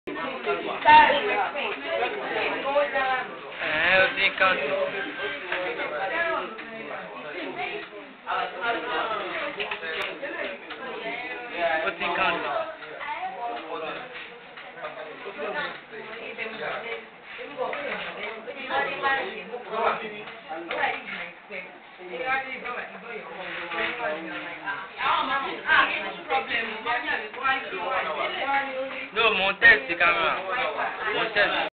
AN INTERVIEW WITH AN ND1 STUDENT ON MATRICULATION DAY. IBRAHIM TYUSUFF OLAWALE
An interview with MIss Salau Abimbola an ND1 student in the Department of Mass Communication (SPTS) , Lagos State Polytechnic